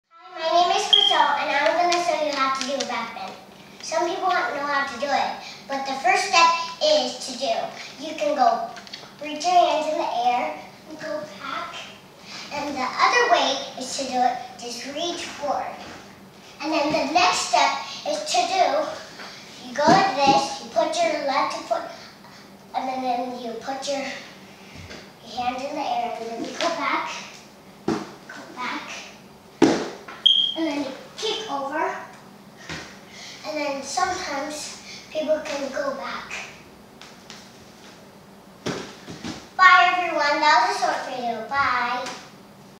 How to do a backbend kickover
For all of you who are dying to know how to do a backbend kick over...this little expert explains it all.

Chriselle says, "it's a short video."

Chriselle is 6 years old, and she loves to dance (her favorite is jazz), tumbling (we're working on her aerial), and she's a great reader!